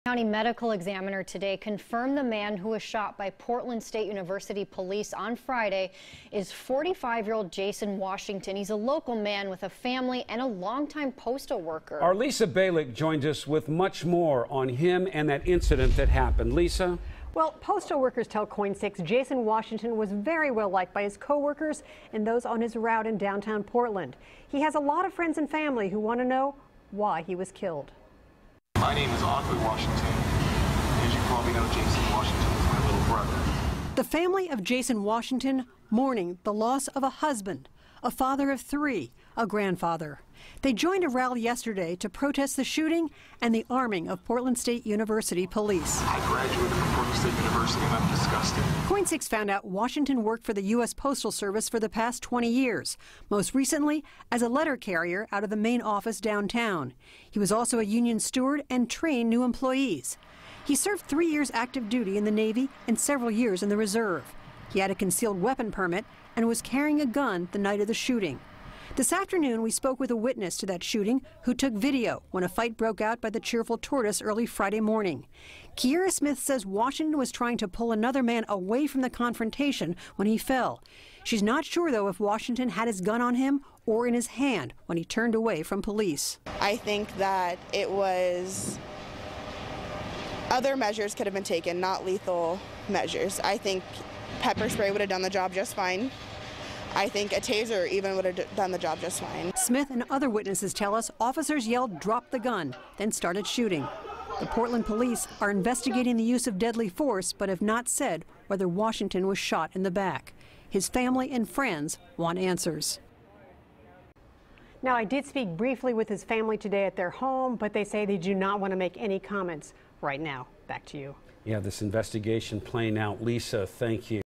Coroner IDs man killed in shooting involving PSU officers
The Multnomah County Medical Examiner identified the person shot and killed on Friday as 45-year-old Jason Washington.